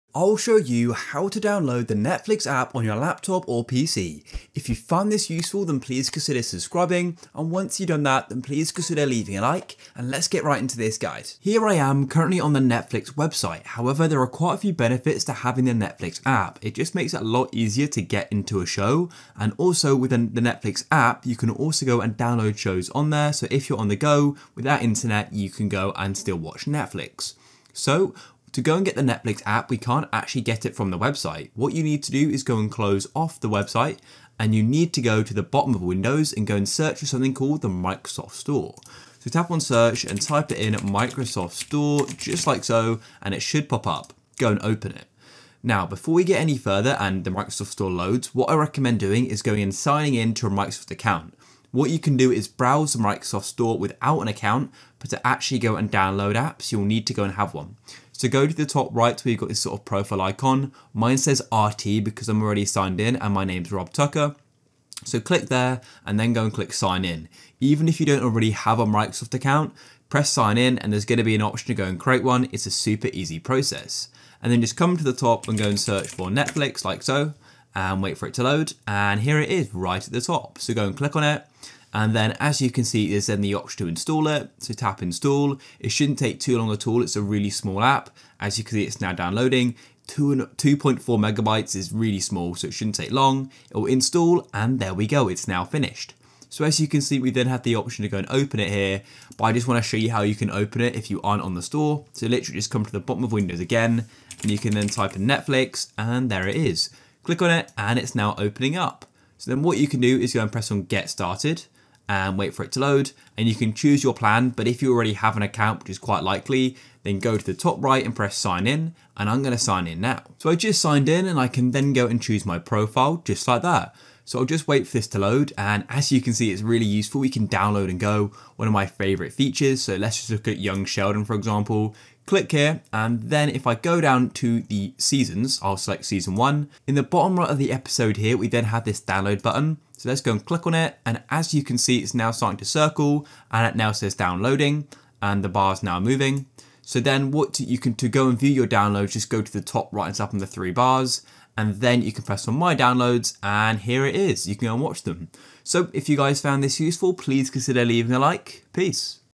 How To Download Netflix On Laptop & PC - Full Guide
I show you how to download netflix on laptop and pc in this video.

GuideRealm